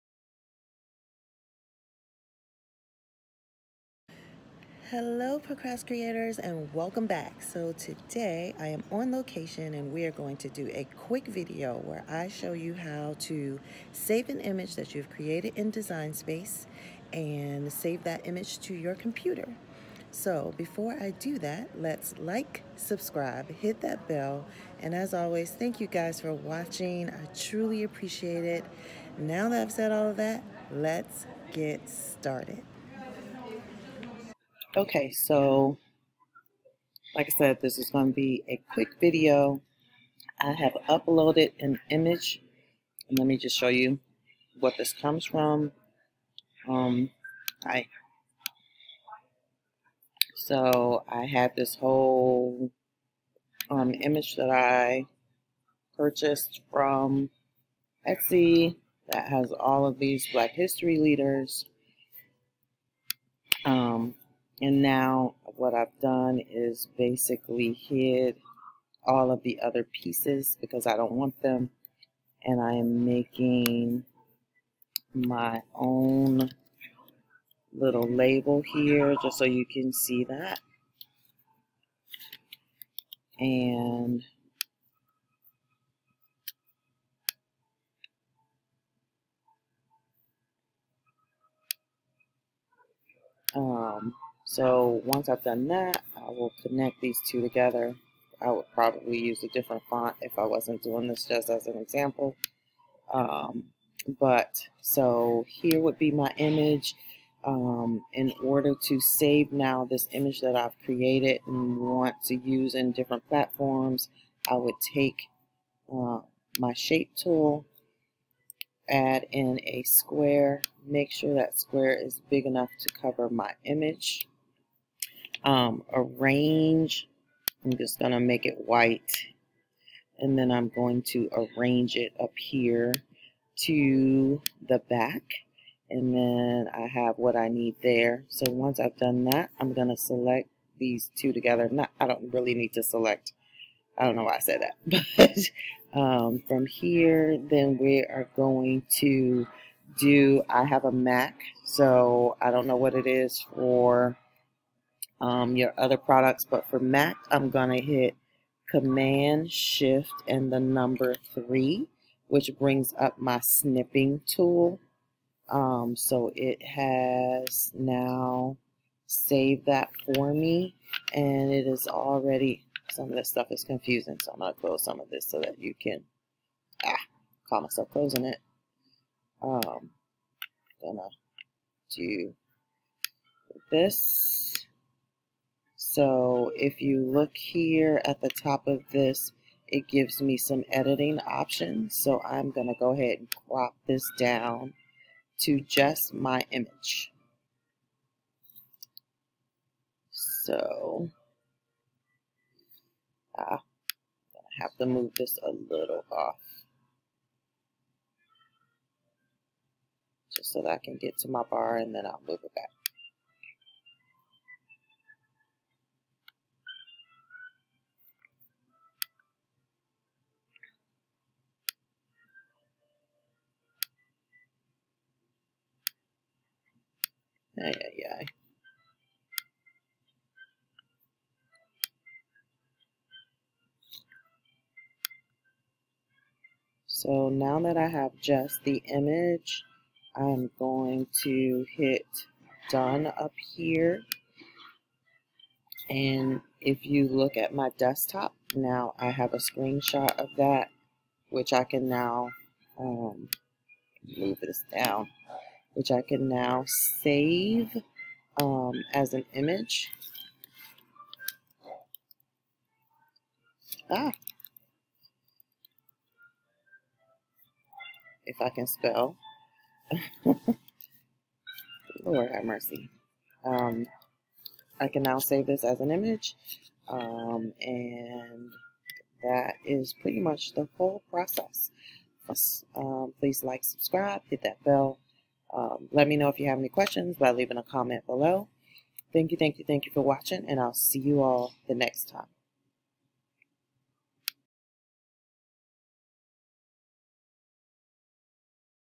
How to Save Your Image from Design Space to Your Desktop
This video is about Exporting an Image from Design Space to Desktop

How to Save Your Image from Design Space to Your Desktop. This video is designed to help those that create an image that they want to use in other formats.This video will show you how to save your image to your desktop.

HTV Vinyl
Vinyl Sheets
Mason Jars

Etsy Shop

About ME
Wife, mother of two sons, educator, author, creator, blogger, student, and grateful human.